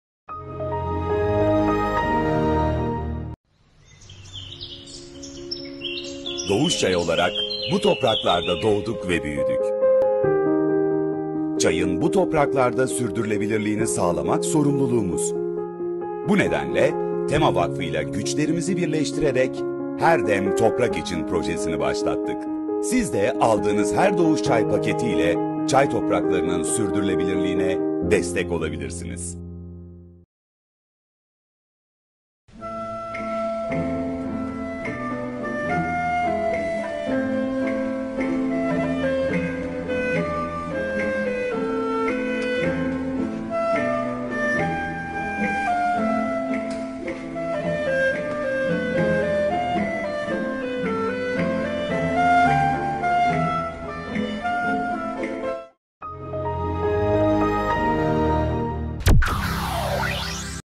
Toprak Sergen Doğuş Çay Tema Vakfı Reklam Filmi
Her dem toprak için ❤ Reklam seslendirmenin keyfi bu özel projelerde ❤❤❤ tebrikler @doguscay @temavakfi